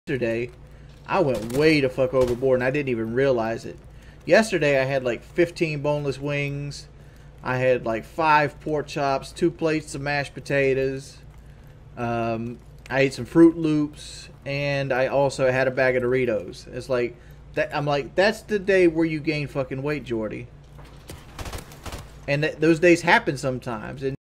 Wingsofredemption Describes His Typical Day of Eating
Richard “Jordie” Jordan reveals the secret to his physique in this rare behind the scenes look at his diet program